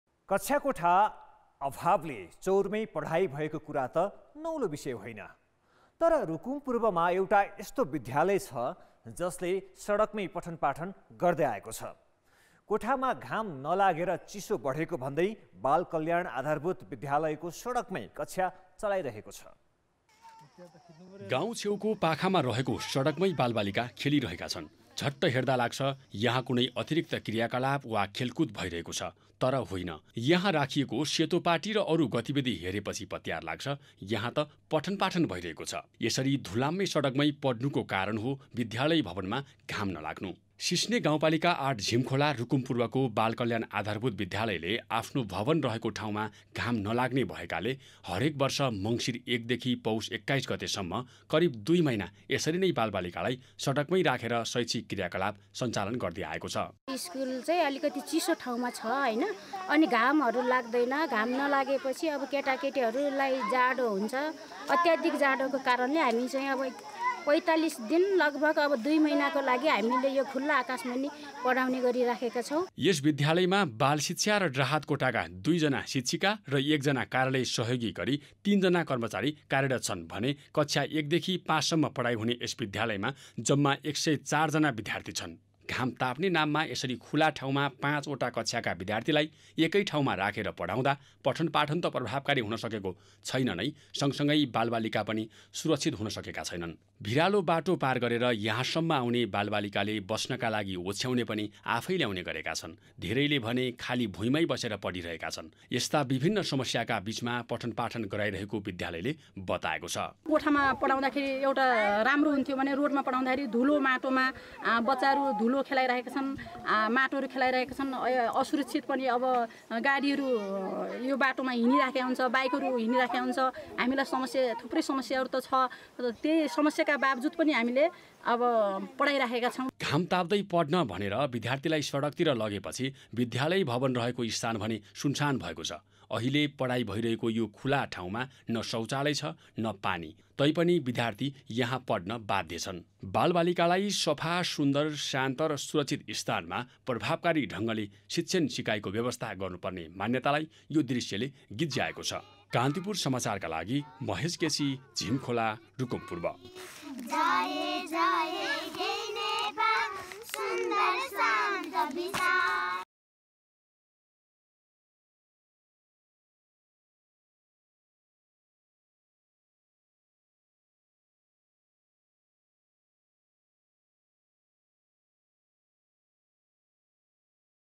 बालबालिकालाई सडकमा राखेर अध्यापन !
कक्षाकोठा अभावले चौरमै पढाइ भएको कुरा त नौलो होइन । तर, रुकुम पूर्वमा एउटा यस्तो विद्यालय छ, जसले सडकमै पठनपाठन गर्दै आएको छ । कोठामा घाम नलागेर चिसो बढेको भन्दै बालकल्याण आधारभूत विद्यालयले सडकमै कक्षा चलाइरहेको छ ।